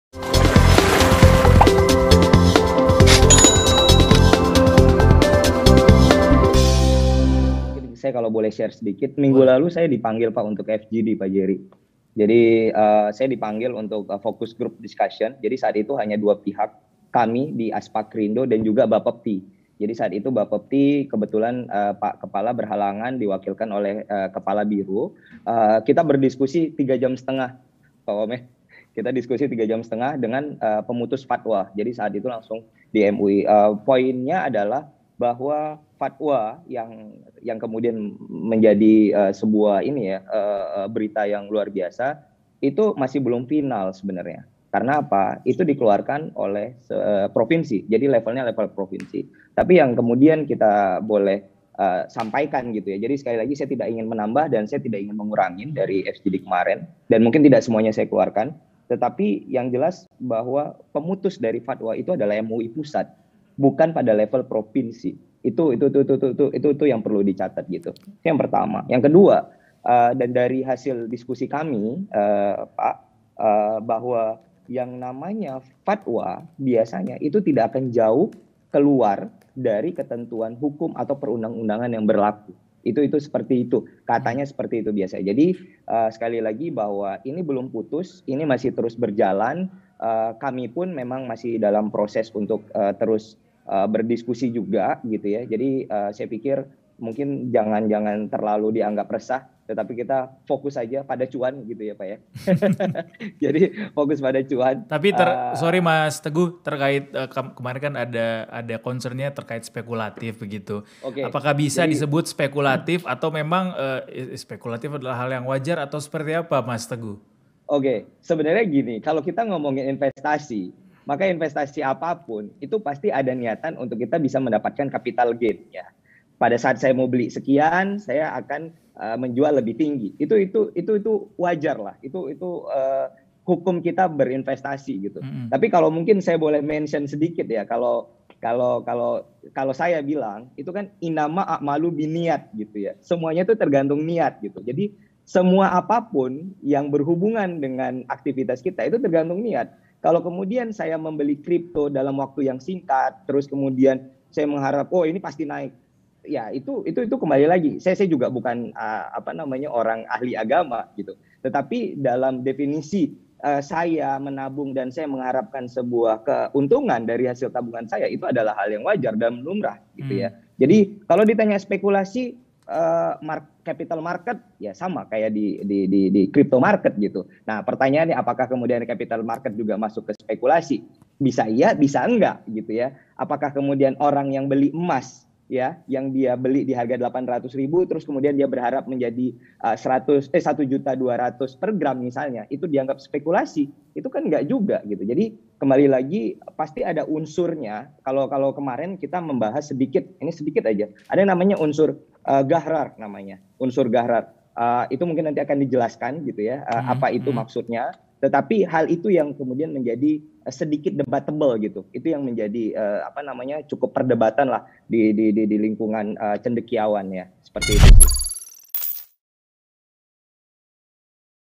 Respon Pedagang Soal Halal Atau Haram Kripto
Asosiasi Perdagangan Aset Kripto Indonesia (Aspakrindo) mengungkapkan sedikit hasil pembahasan halal atau haram kripto bersama Bappebti. Diskusi itu juga melibatkan pemutus fatwa MUI.